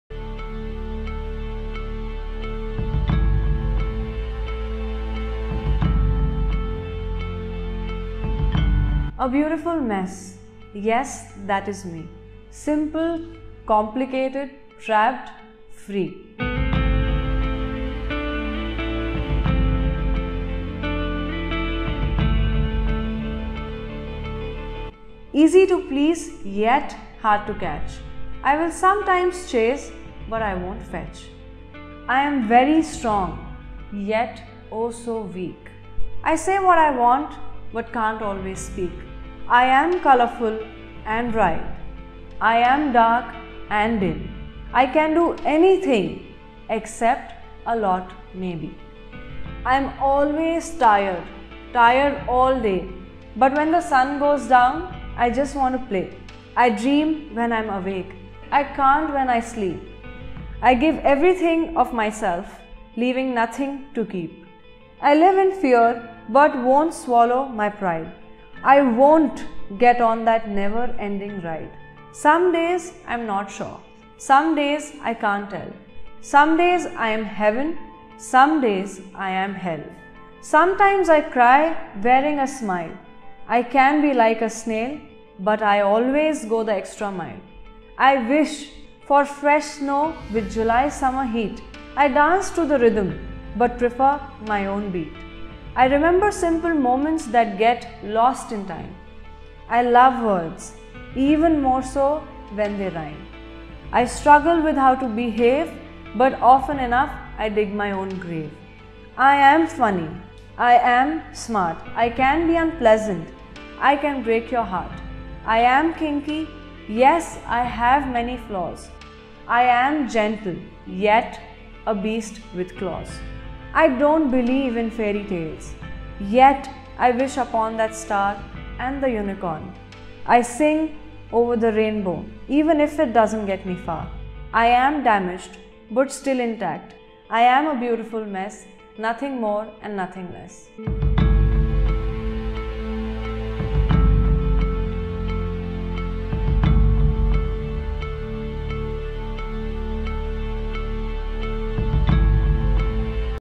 Spoken Word Poetry | "A BEAUTIFUL MESS" - GOPICA | Sad Poetry | Slam Poetry Recital | Hyderabad
What's more enchanting when you know yourself completely and yet you know you are in a mess... Here's a lovely poem by Gopica and the beautiful mess she is.

Direction: KSS
Cinematography/Editing: Vish